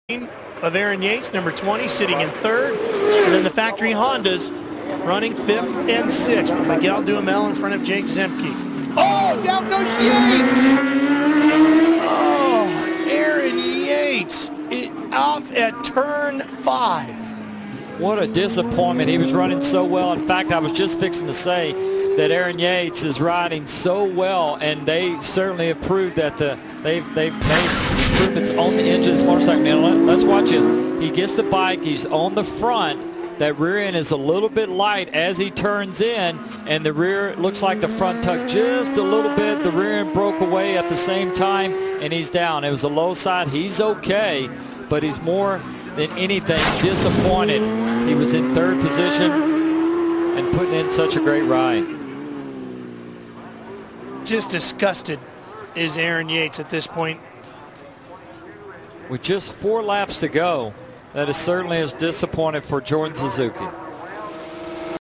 AMA 2007 - Road America - Aaron yates crash
AMA 2007 Rnd05 Road America Race1 - Aaron yates crash down wile riding 3rd